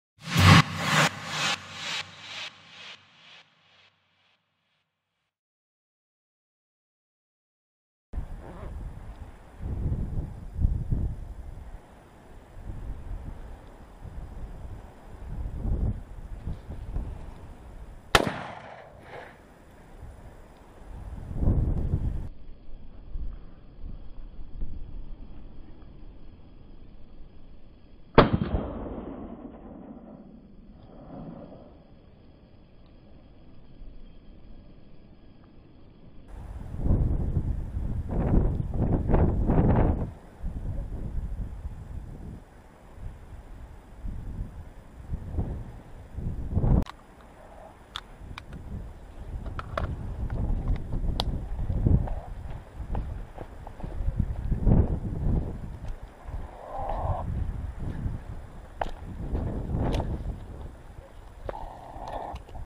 VIDEO DE CAZA A JABALI 🐗 - Montería en 2019
En este vídeo de caza a jabalí en montería os traemos un lance en el que podemos apreciar la astucia de estos macarenos cuando se levantan zorreados y buscan los pasos mas sigilosos para emprender su huida sin ser vistos ni oídos. Enhorabuena al cazador!